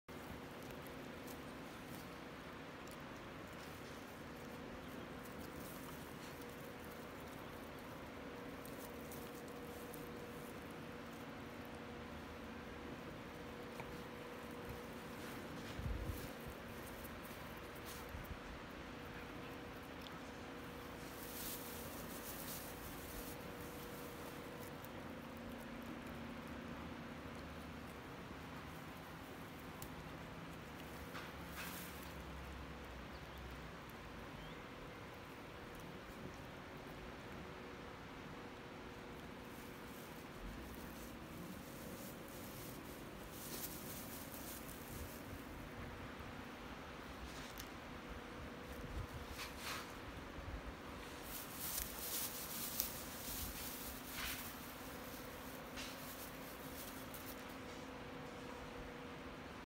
Dog high on weed
Dog high on weed after eating